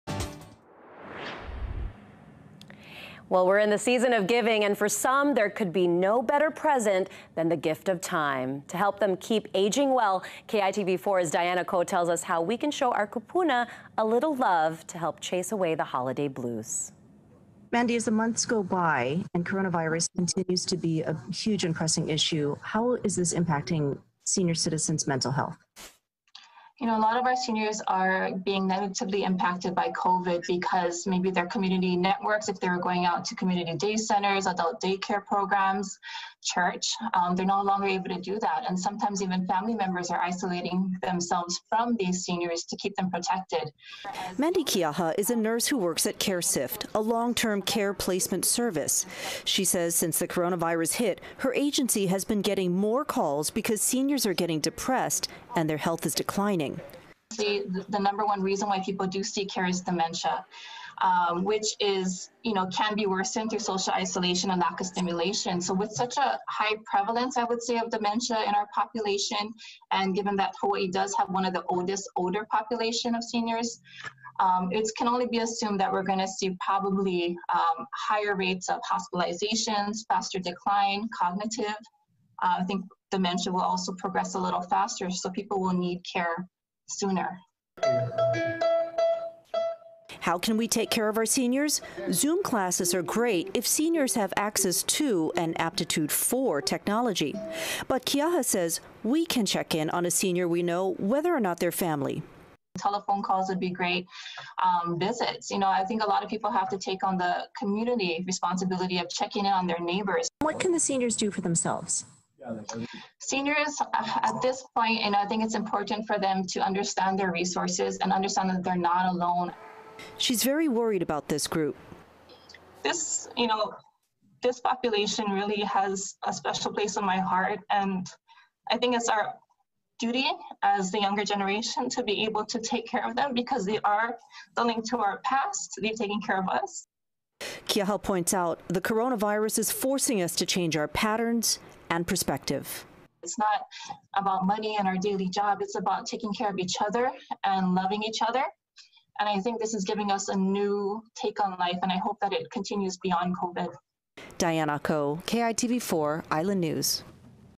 20 12 22 KITV Aging Well COVID 19 exacerbates holiday depression for seniors
Aging Well: COVID-19 exacerbates holiday depression for seniors

Geriatric professional says the community needs to help seniors suffering from loneliness and social isolation.